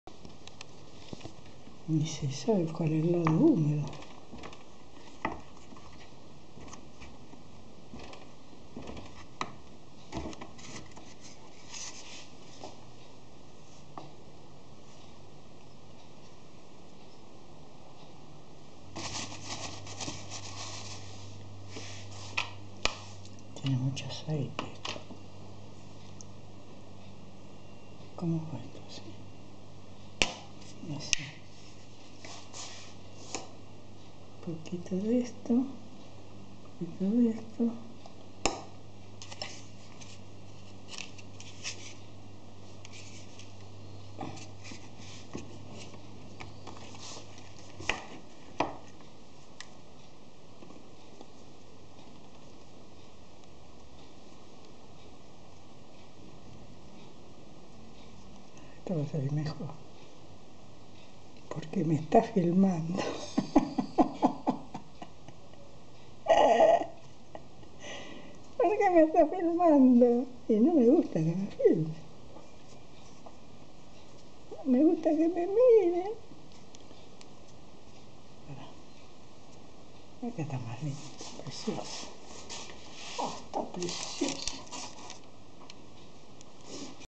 2014 08 19 Empanadas Film 019
Mise en forme d'une empanada argentina, viande,oeuf cuit dur, olive, et oignons verts.